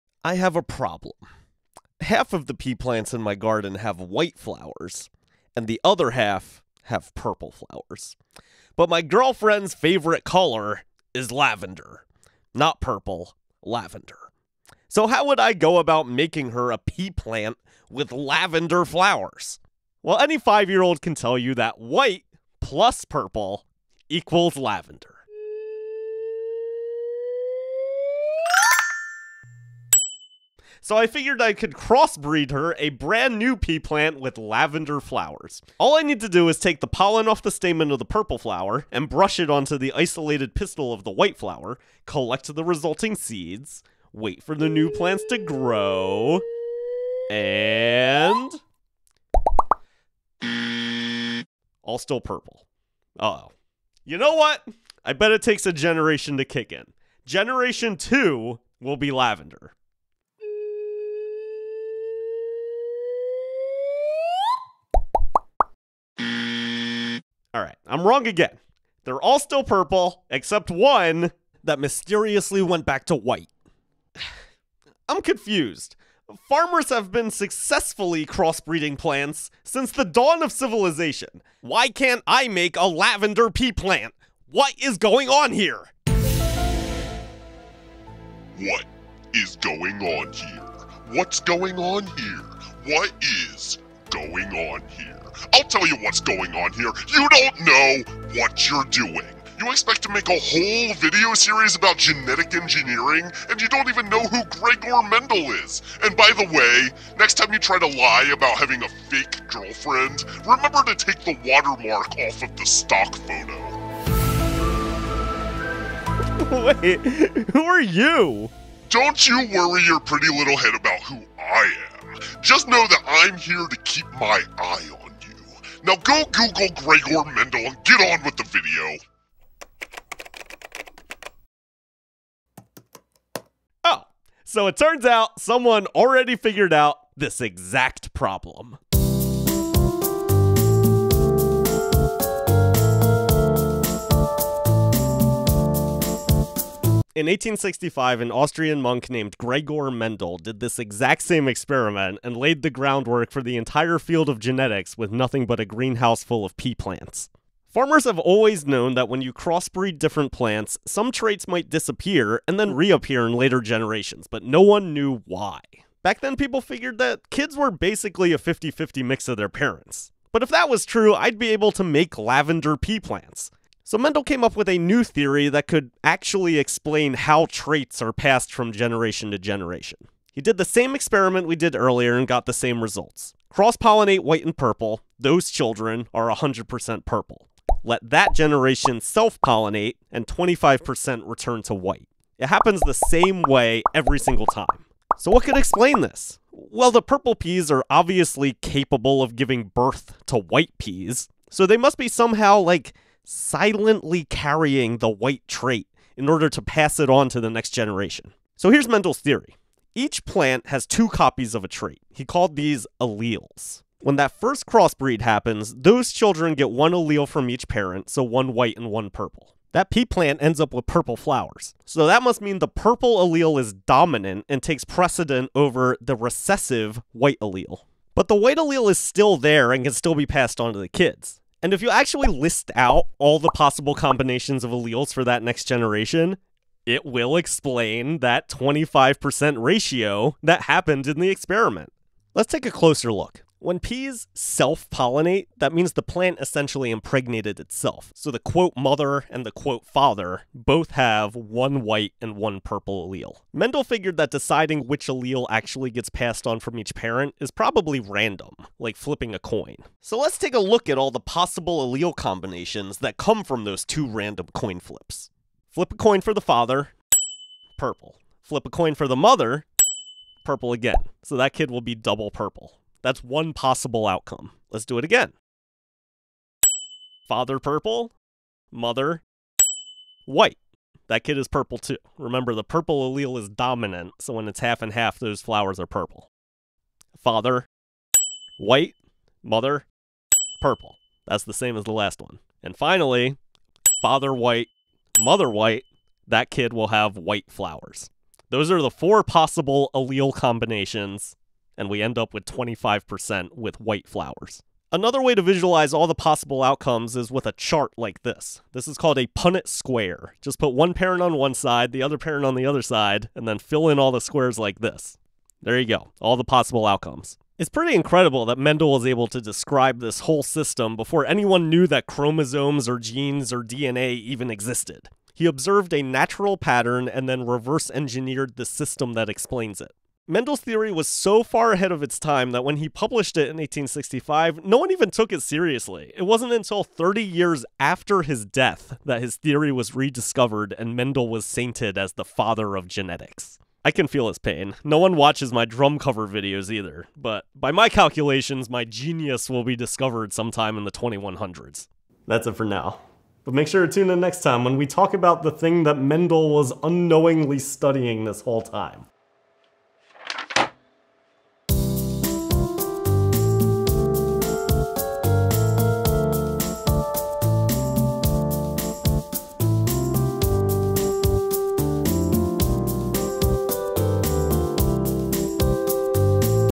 Gregor Mendel and the Dawn of Genetics
WHY CAN'T I MAKE A PEA PLANT WITH LAVENDER FLOWERS?

Part of our series, CRISPR for the Classroom:
Written and Directed by Frank Hegyi
Science by Dr. Jake Daane and Dr. Bill Detrich
Starring Frank Hegyi, Dr. Jake Daane, and Jeriyla Kamau-Weng
w/Special Guest Dr. Pyramid Eye, Esq.

Supported by U.S. National Science Foundation Grant Opp-1955368